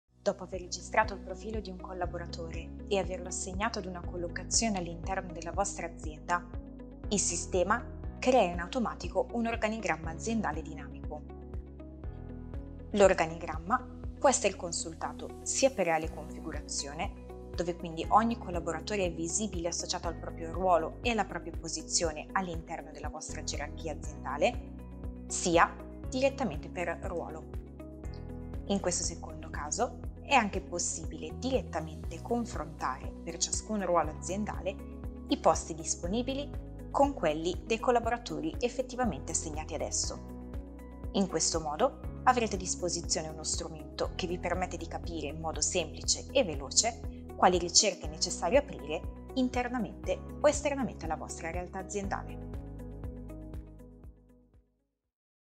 Mploy | Organigramma per ruolo e per collaboratore
Una volta effettuato l’onboarding dei candidati, il sistema crea in automatico un organigramma aziendale dinamico, consultabile sia per ruolo (visibilità sulla sola struttura, senza alcun riferimento ai singoli collaboratori) sia per reale configurazione (ciascun collaboratore viene mostrato, all’interno dell’organigramma, associato al proprio ruolo specifico).